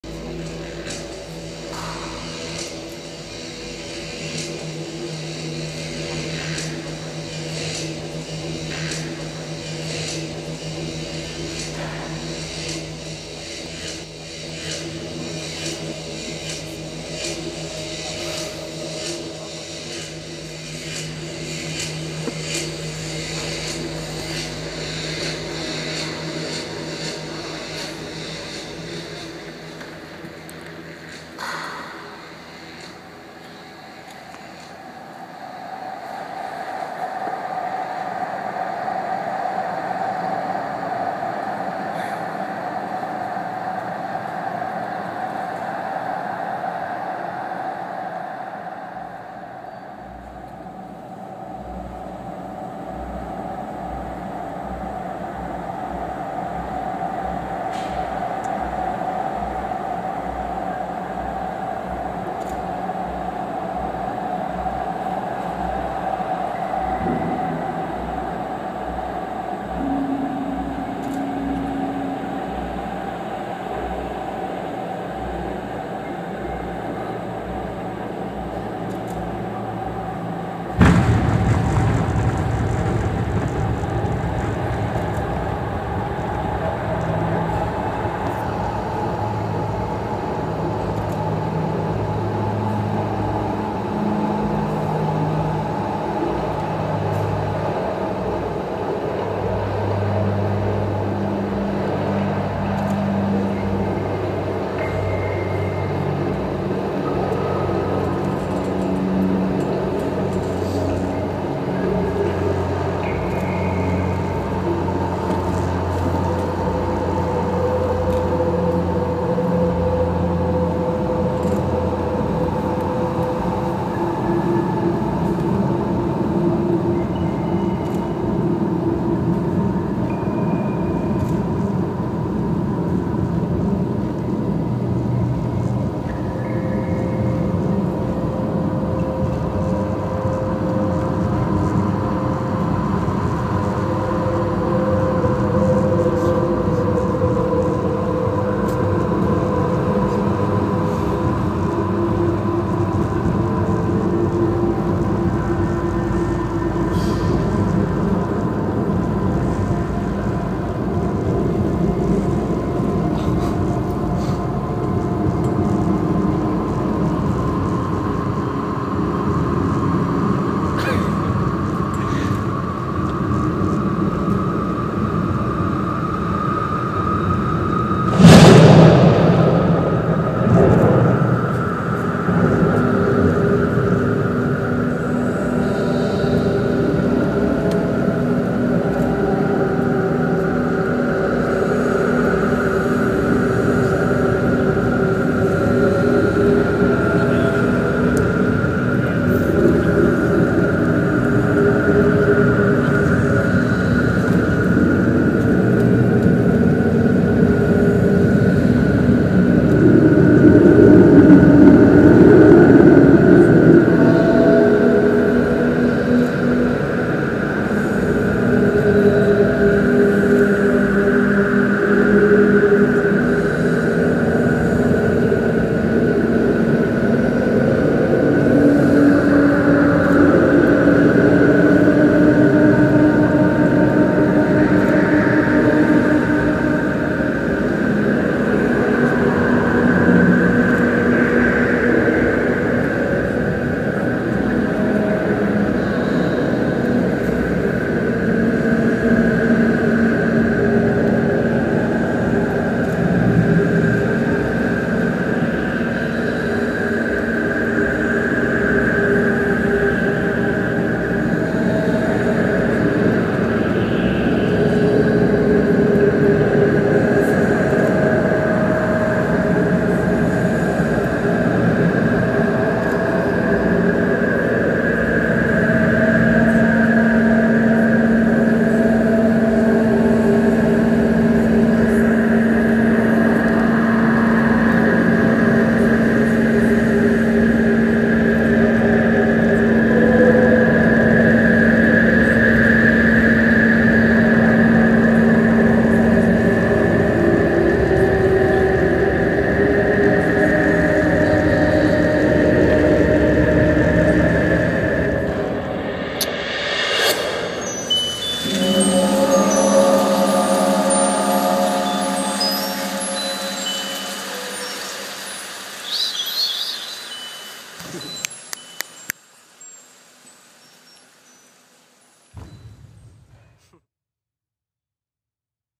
ASCENT
Shot on my iPhone 4s

Camera: Chris Rogy
Edit: Chris Rogy
Zen Master: Sue Ngo

The Ascent is a transcendent journey that allows individuals to levitate 30-plus feet upward into the air through fields of intense sound and lighting via the power of their own focus and meditation.

Wearing an EEG brain-wave sensor and a harness, a rider steps onto the launch pad where an attendant clips her into the flying cables. The room is pulsating with low frequencies generated by her brainwaves in real-time. As an audience watches, the rider's concentration begins to lift her into the air. A storm of stimuli conspires to distract her from reaching her goal: levitating into "transcendence" and "winning" by unleashing a climactic, firework-filled, grand-prize explosion, immortalizing the rider in an ephemeral blinding moment of super-human glory.

Part game, part iconic spiritual simulation, mental gauntlet meets hero's journey, The Ascent blurs the lines between ambition and surrender, and delivers a playfully spectacular, mind-altering take on the intersections between humans and machines, transcendence and technology.